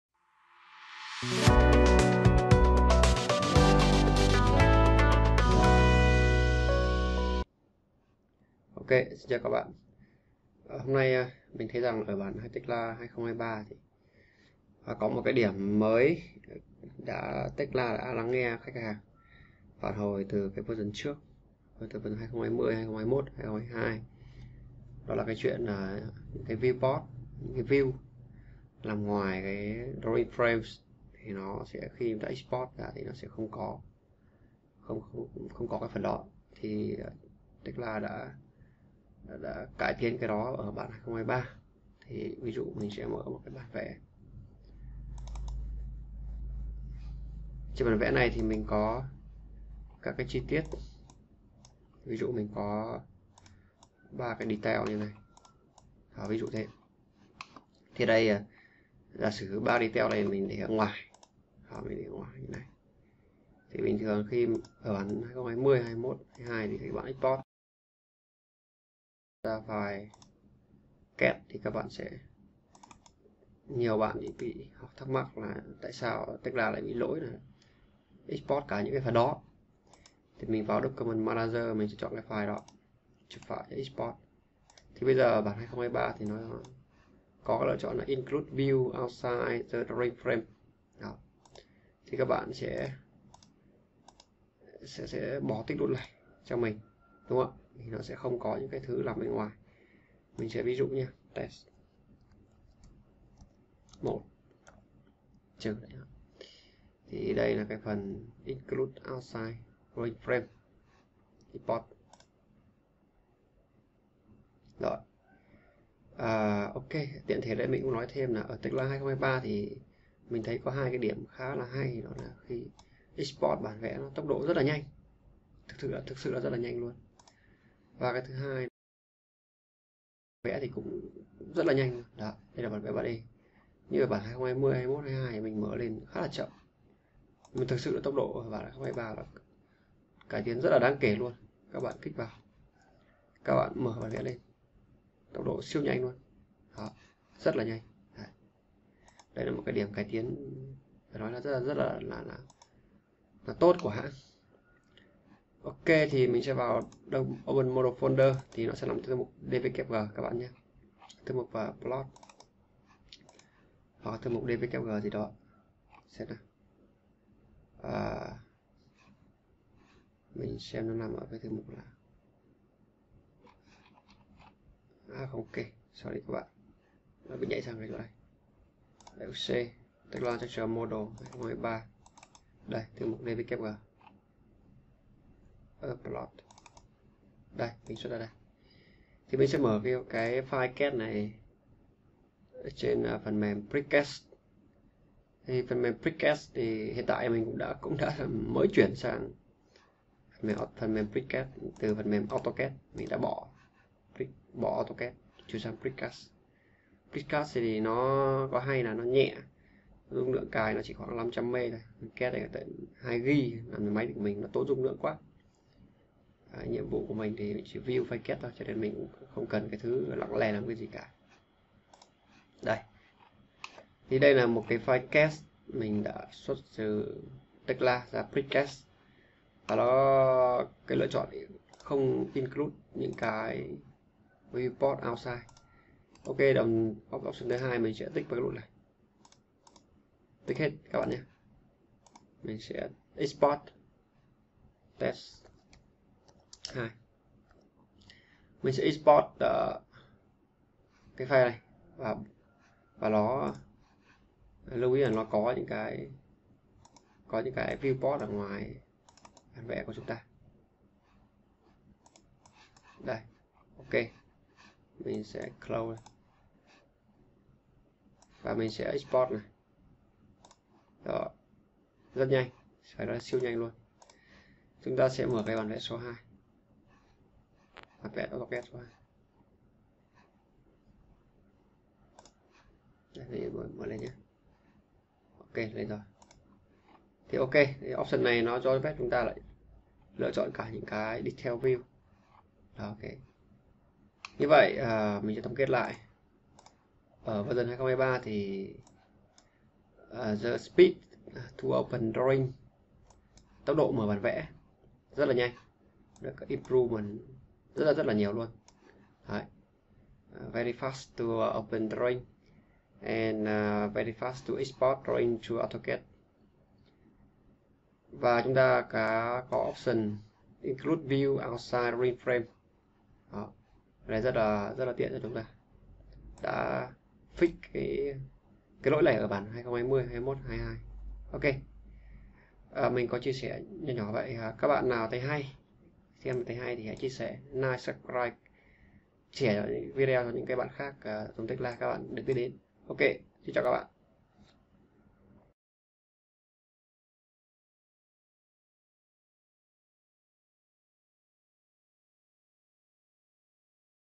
Tekla TIP #87 Tekla 2023 Include view outside the drawing frame
Hướng dẫn một tính nắng mới ở Tekla Structures 2023, giúp người dùng khi export bản vẽ ra file CAD. Có thể chọn hay không xuất các đối tượng nằm ngoài khung bản vẽ.